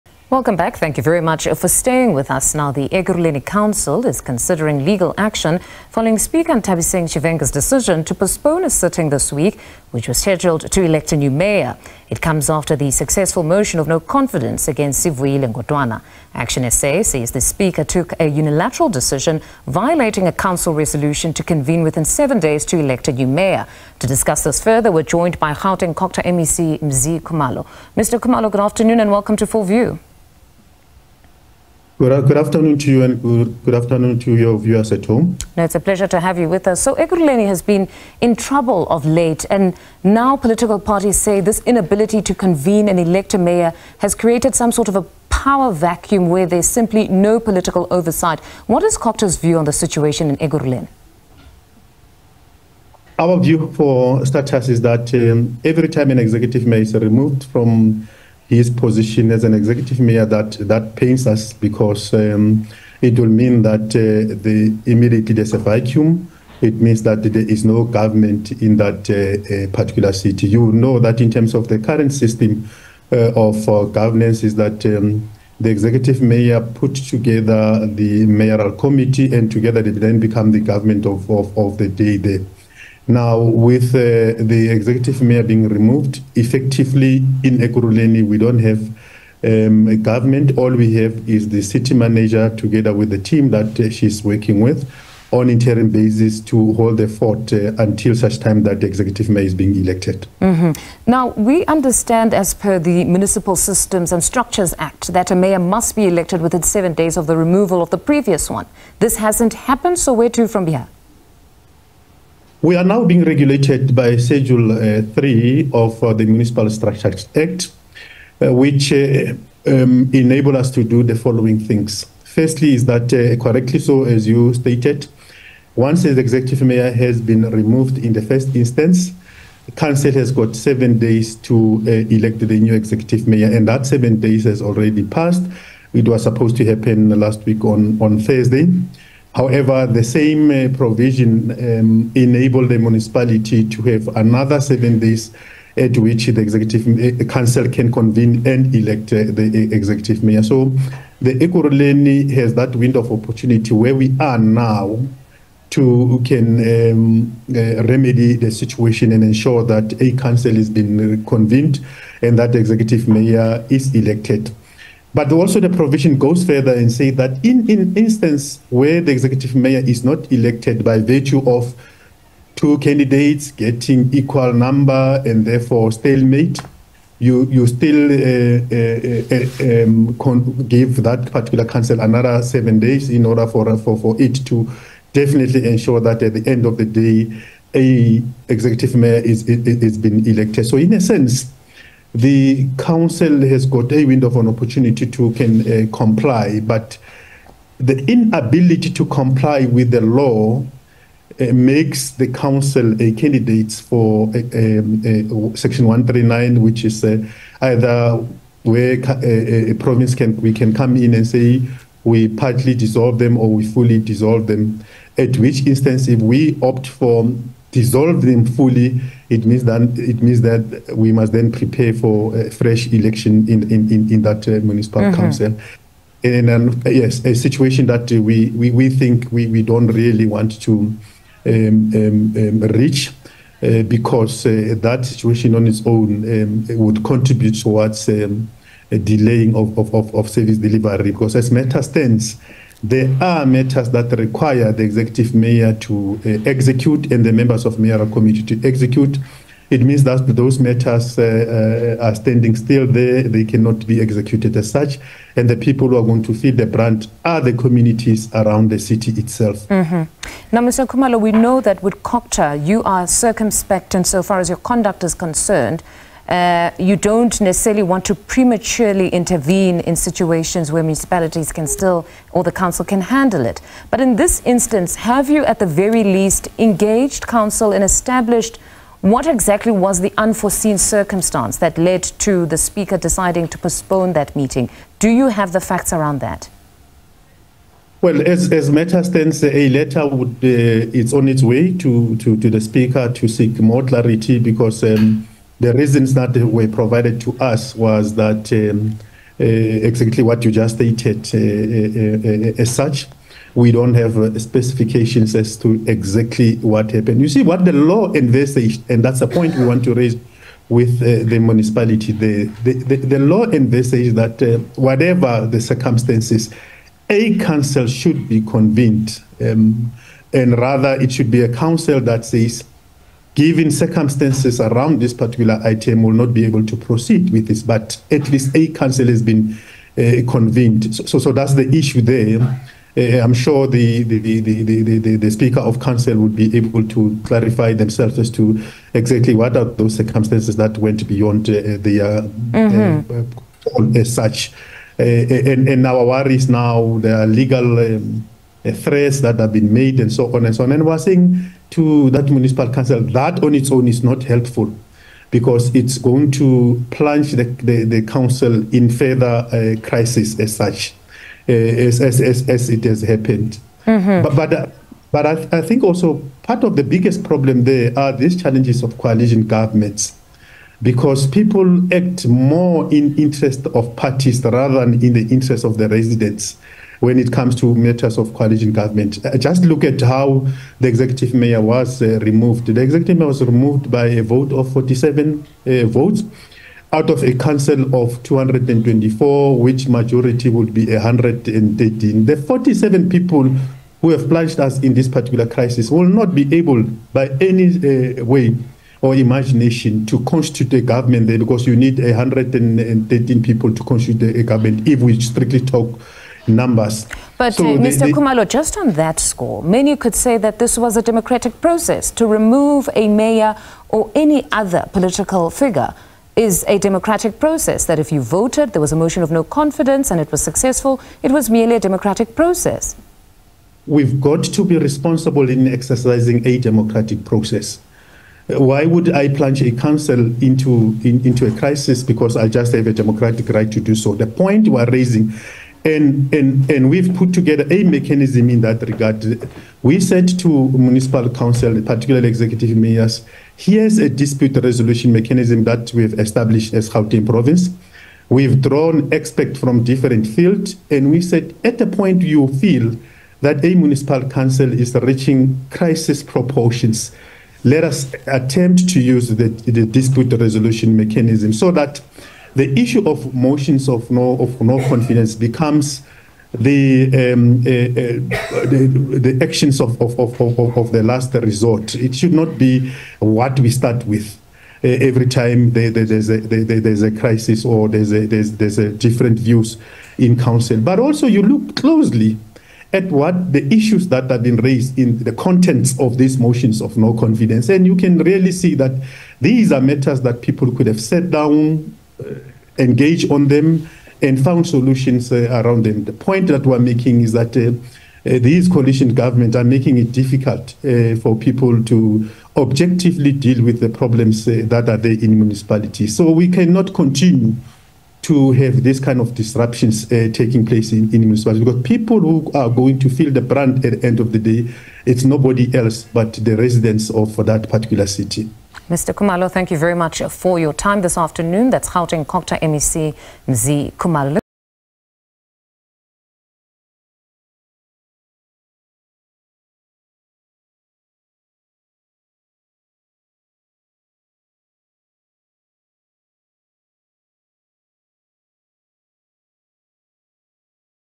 2024 Elections | Ekurhuleni council to elect new mayor: Mzi Khumalo
The Ekurhuleni council is considering legal action following speaker, Nthabiseng Tshivhenga's, decision to postpone a sitting this week which was scheduled to elect a new mayor.

It comes after the successful motion of no confidence against, Sivuyile Ngodwana.

ActionSA says the speaker took a unilateral decision violating a council resolution to convene within 7 days, to elect a new mayor.

To discuss this further we're joined by Gauteng COGTA MEC,  Mzi Khumalo